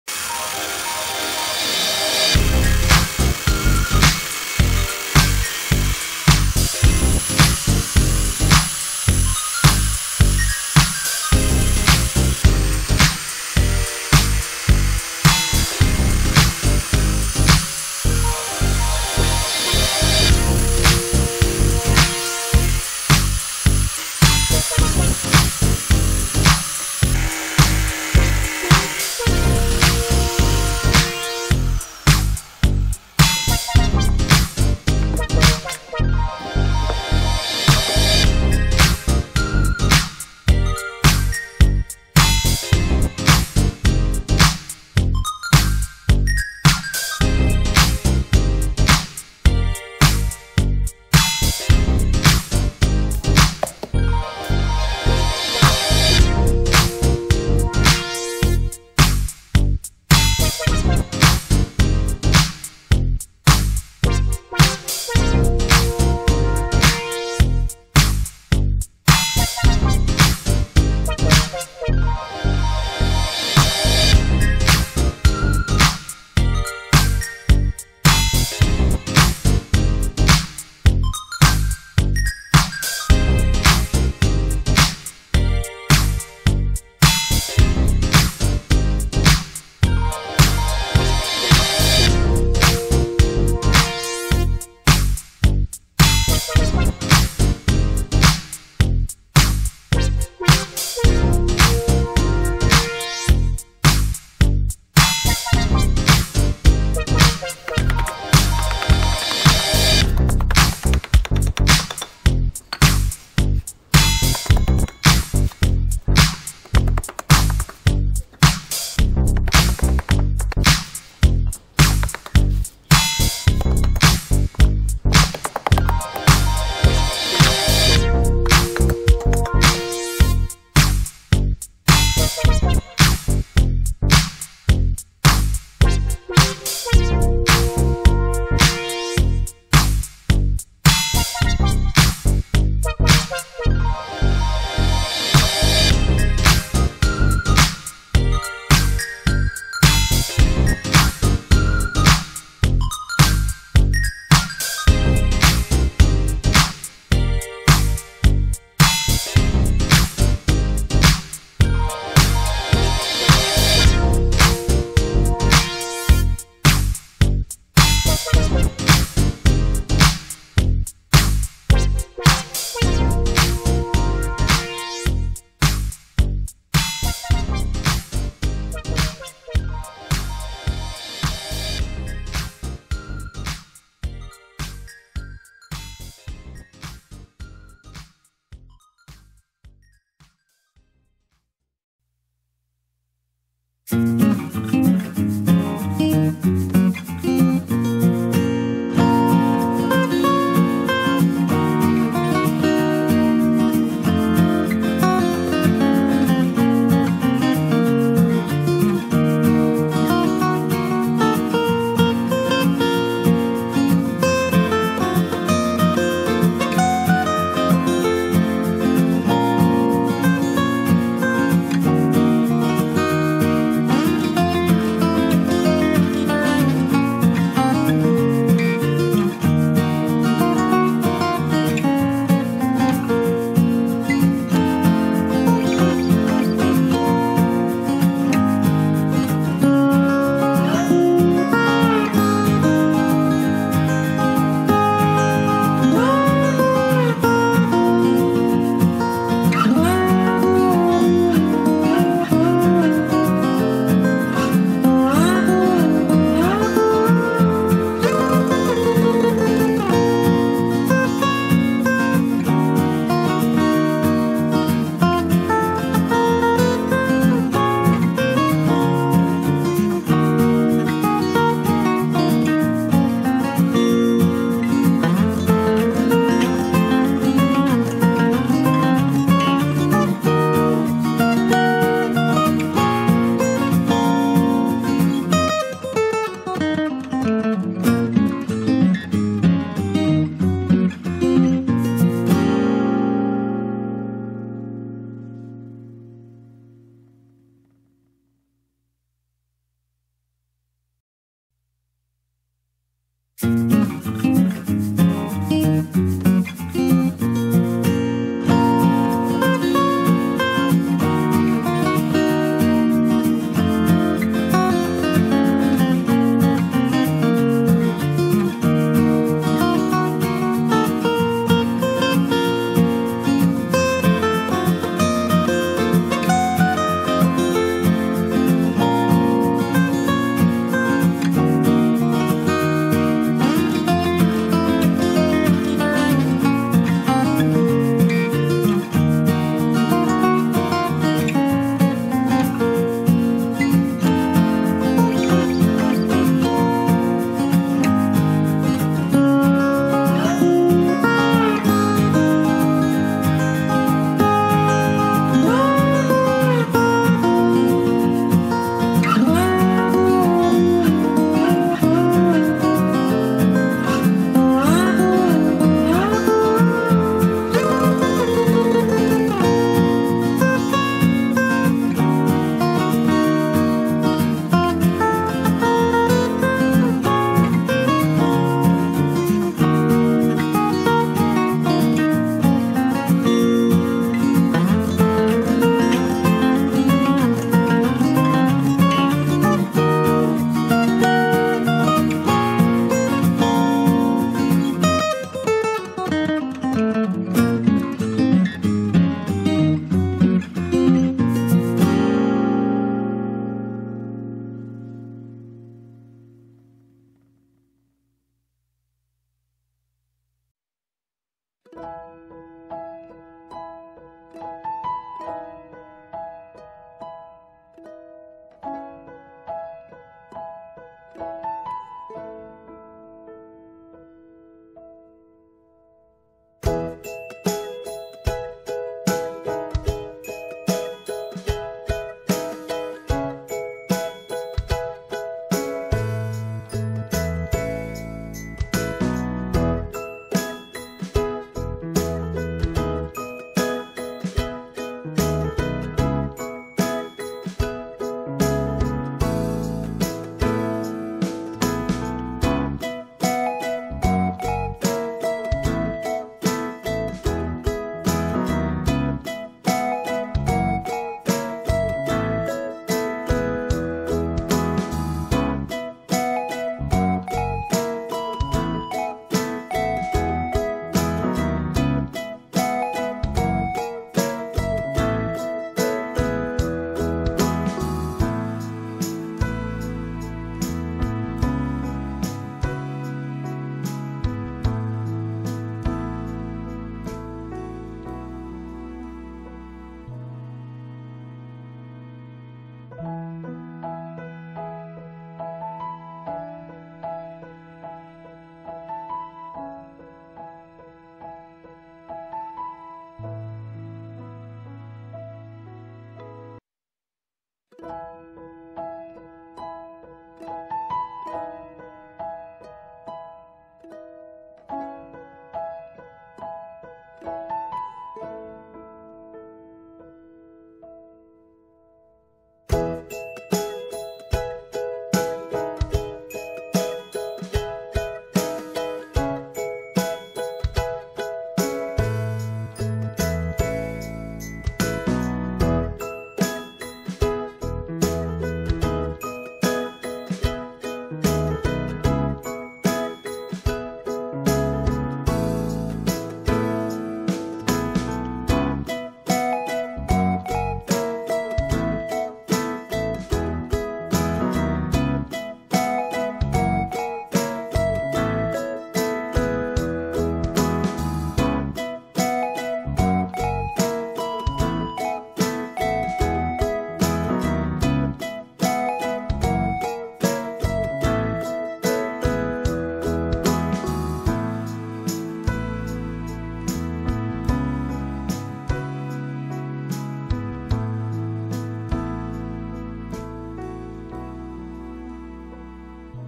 TABLE COFFE AMAZING. DIY outdoor coffee table from broken chairs and cement
Welcome to Vu Dat's official Creative at Home channel.
Let's protect the environment together. By Recycling discarded items into unique and meaningful products for your home.